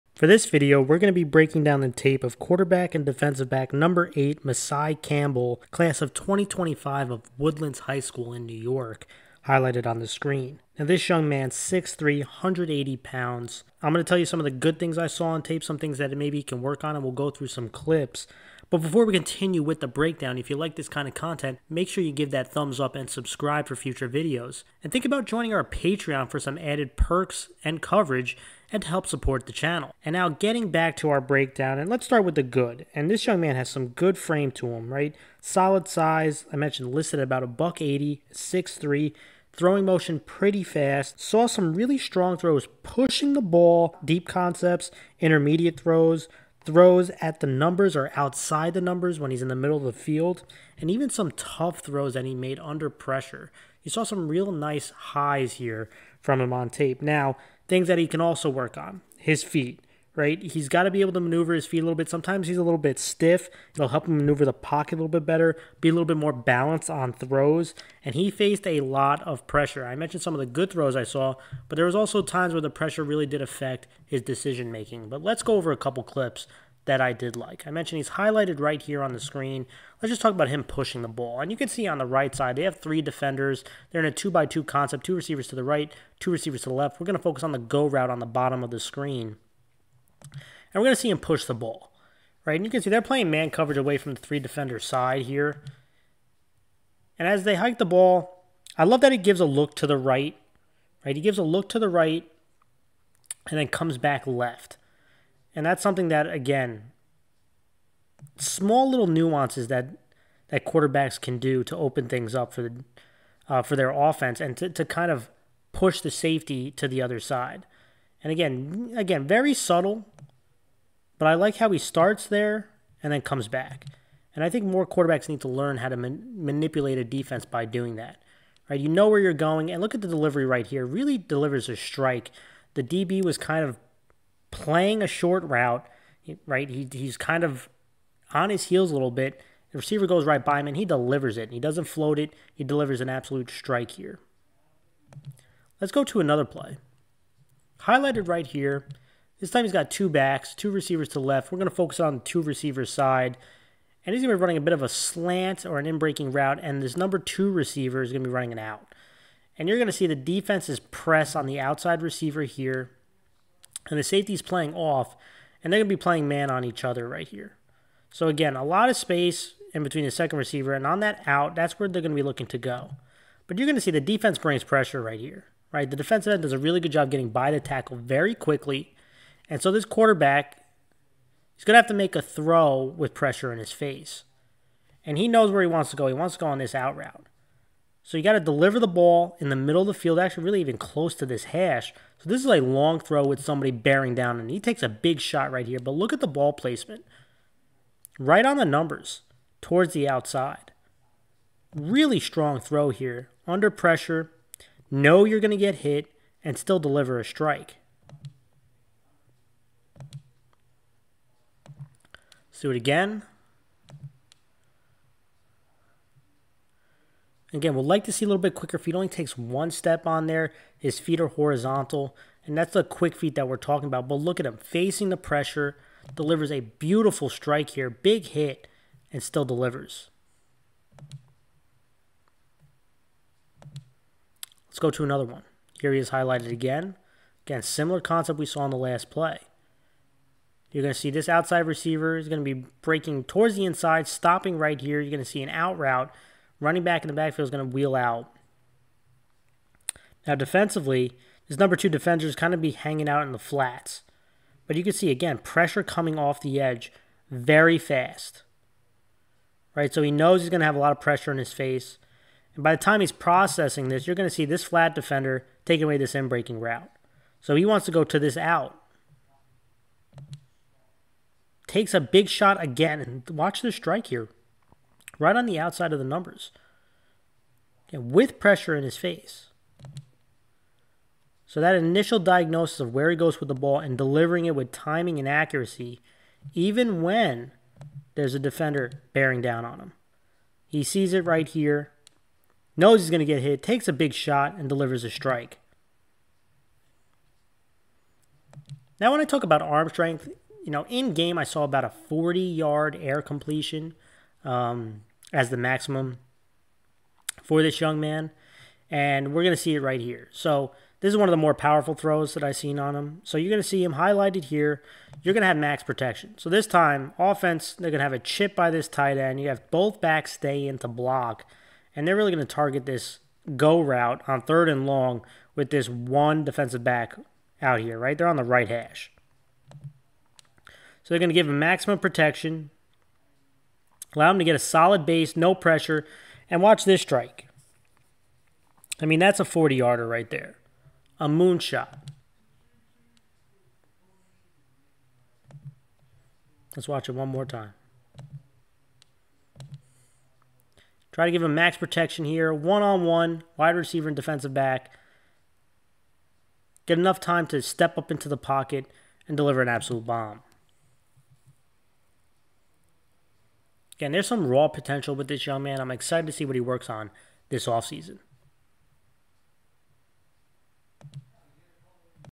Masai Campbell (QB/DB) - Class of 2025
Frank's Film Room - In this video, we break down the football film of #8 Masai Campbell (QB/DB) Class of 2025 of Woodlands HS in NY.